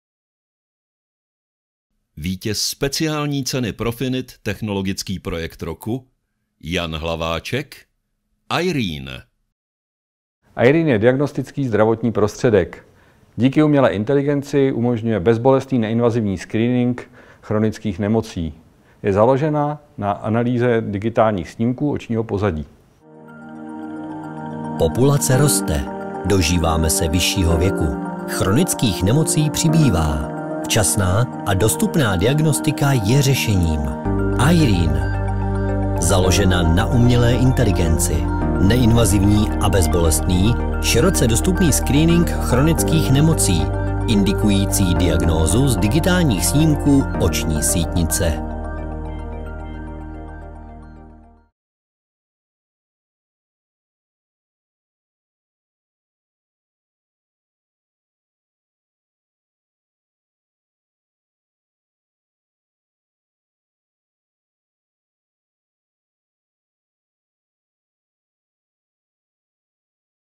Aireen - Vítěz Profinit Technologický projekt roku 2021
Aireen je diagnostický zdravotní prostředek založený na AI / DeepLearning. Poskytuje neinvazivní, bezbolestný screening chronických diagnóz, založený na fundus snímcích sítnice oka. Umožňuje tak včasný záchyt nemocí již během prvotního kontaktu s lékařem. Za méně než 30 sekund, ve 3 jednoduchých krocích nabídne vysokou citlivost a detailní diagnostický výstup. Aireen již úspěšně provozována na několika odborných pracovištích.